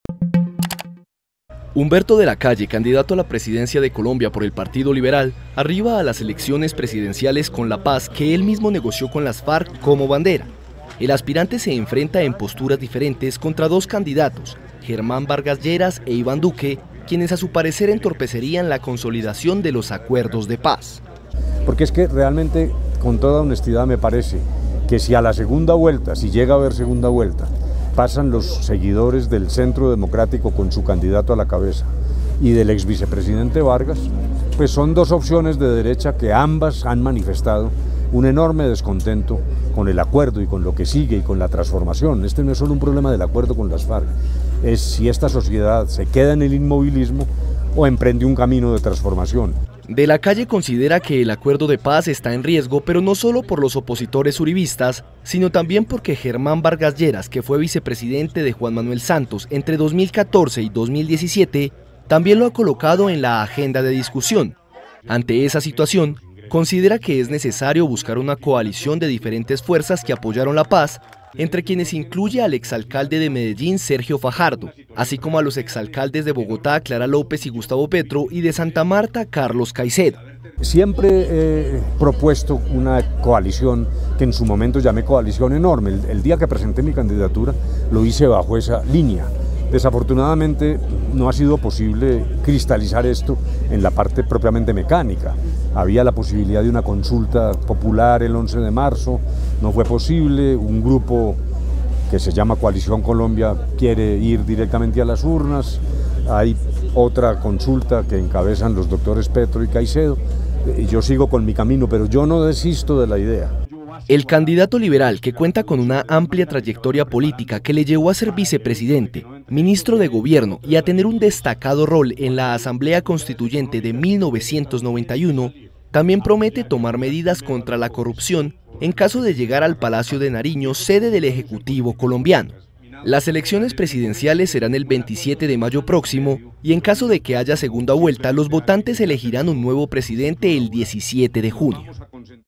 De la Calle aspira a la Presidencia con la paz de Colombia como bandera
Bogotá, 29 ene (EFE),(Imágenes: Juan Pablo Alemán).- Humberto de la Calle llega a las elecciones presidenciales de este año no sólo como candidato del Partido Liberal Colombiano, sino también como el aspirante que busca garantizar la paz con las FARC, que él mismo negoció, frente a los dos nombres de la derecha: Germán Vargas Lleras e Iván Duque.

IMÁGENES:
RECURSOS Y DECLARACIONES DEL CANDIDATO A LA PRESIDENCIA DE COLOMBIA HUMBERTO DE LA CALLE.